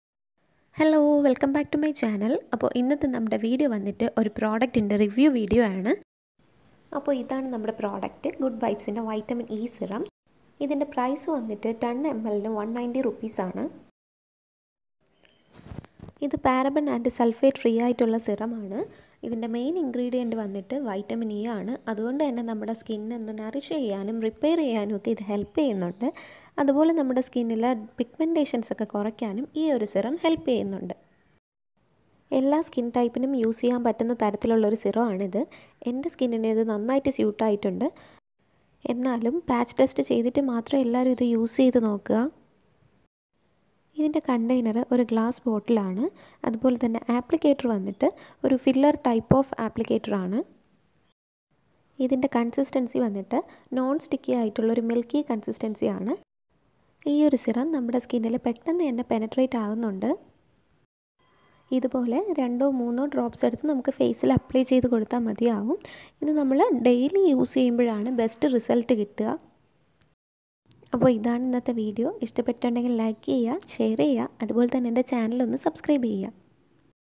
good vibes ന്റെ വിറ്റാമിൻ ഇ serum ശരിക്കും നല്ലതാണോ 🤔|| malayalam review
hello frnds
 this video consist if a product review

product
good vibes vitamin e serum